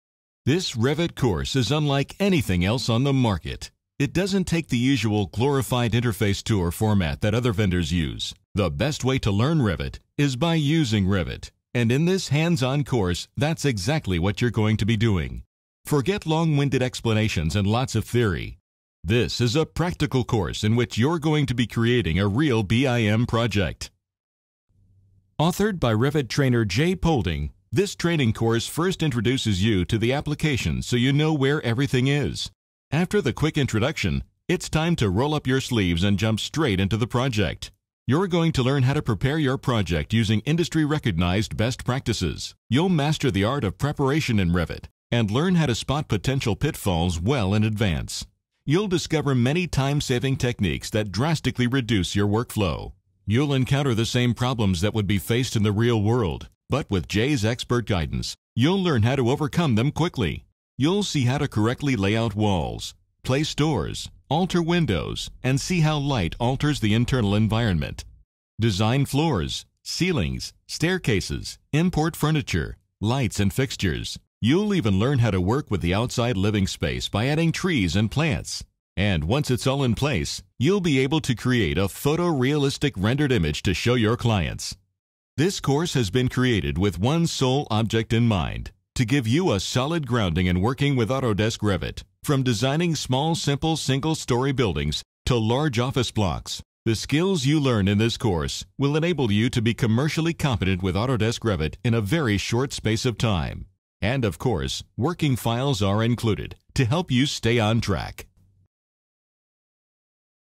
Revit 2011 Tutorial - How to Learn Revit the Easy Way
Want all of our free Revit videos?

Learn how to learn Revit 2011. A easy to follow tutorial video that breaks Revit down into easy to follow stages. YouTube